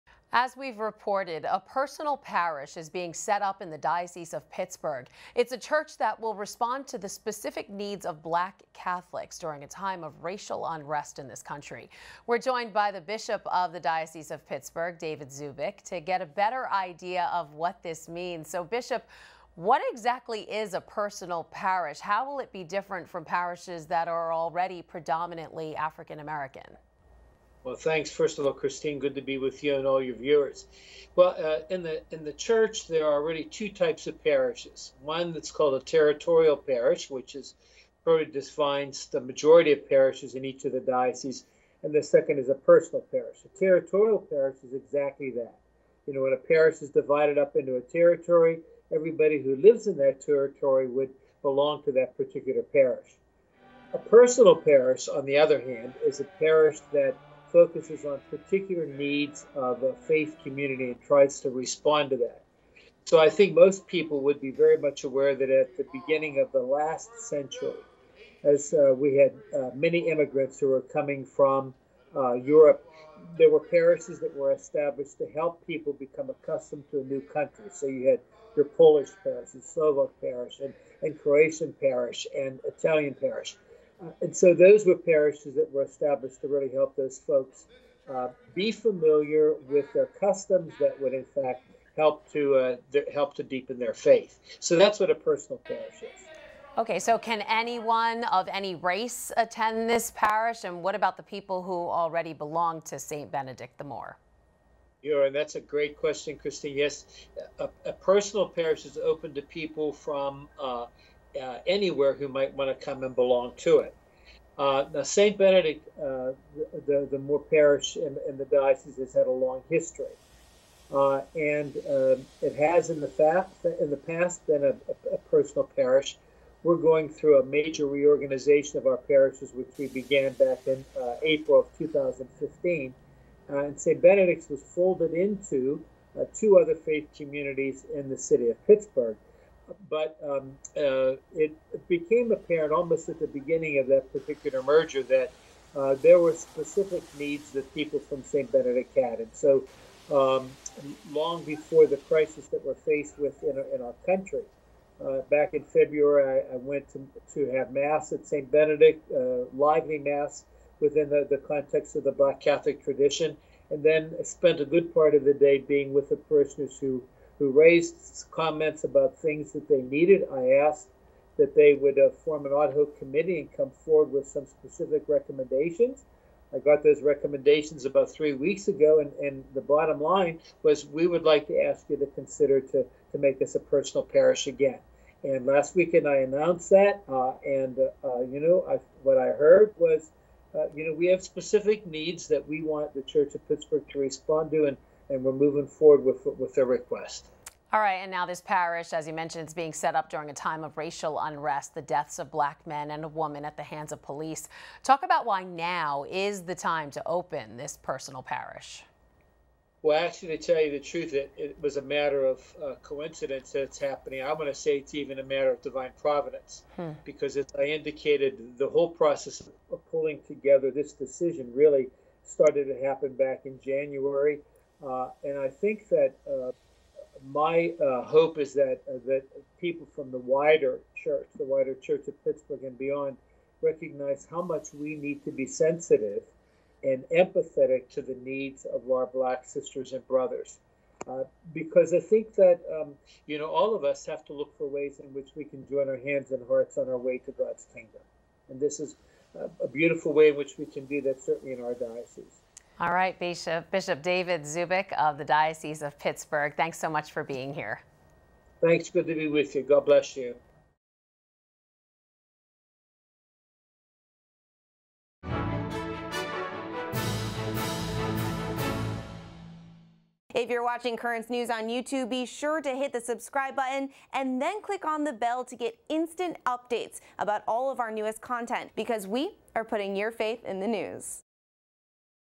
Bishop David Zubik Shares Why Now Is the Time for a New Personal Parish in Pittsburgh
A personal parish is being set up in the Diocese of Pittsburgh, Pennsylvania. It’s a church that will respond to the specific needs of Black Catholics during a time of racial unrest in the U.S.Currents News is joined by Bishop David Zubik to get a better idea of what this new church will mean for his diocese.